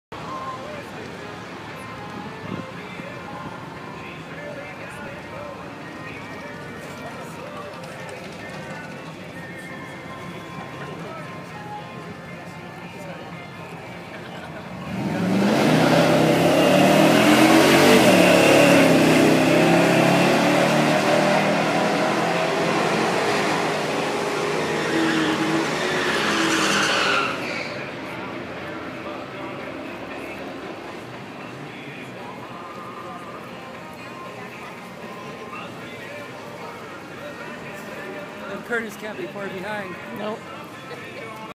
the old jaguars first run
2004 xkr portfolio convertible.   4..2 litre.   ran 14.4. no burnout, no staged rpm.
no sport mode. just ease off the line on green.